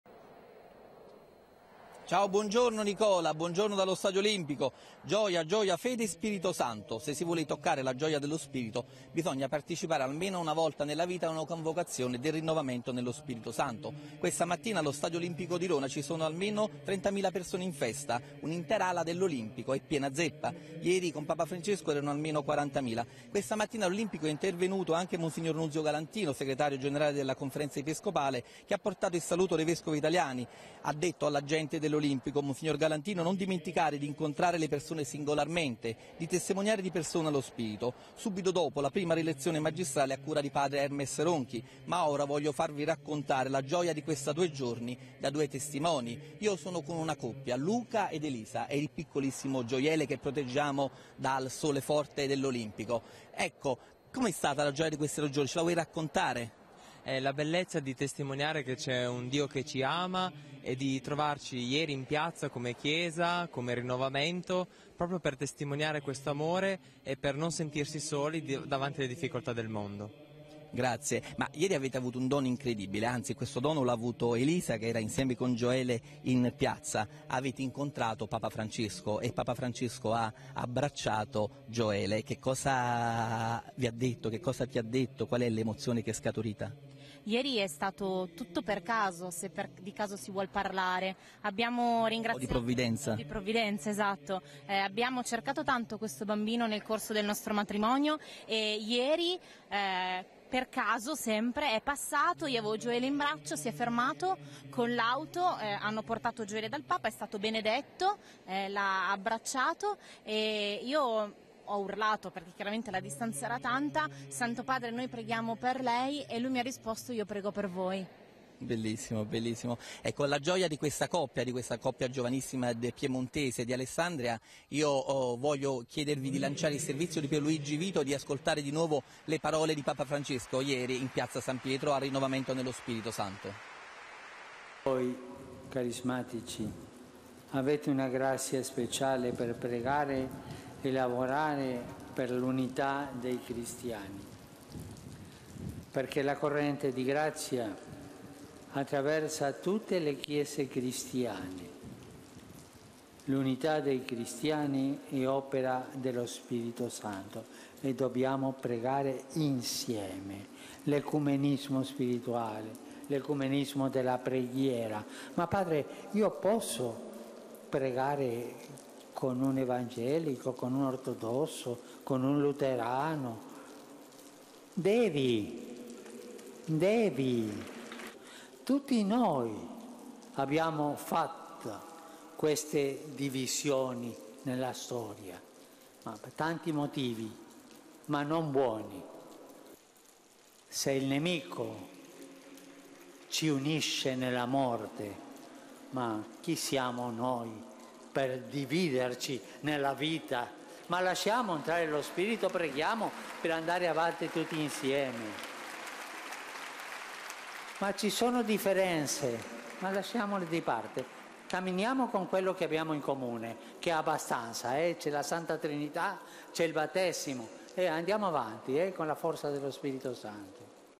Dopo l'abbraccio di Papa Francesco al popolo di RnS prosegue la 38a Convocazione
L’ABBRACCIO COL PONTEFICE CHE LI AVEVA ESORTATI A COLTIVARE L’ECUMENISMO SPIRITUALE. PARLIAMO DELLA 38ESIMA CONVOCAZIONE NAZIONALE DEL RINNOVAMENTO NELLO SPIRITO. L’INCONTRO PROSEGUE OGGI ALLO STADIO OLIMPICO DOVE ANDIAMO SUBITO. IN COLLEGAMENTO PER NOI LUIGI FERRAIUOLO.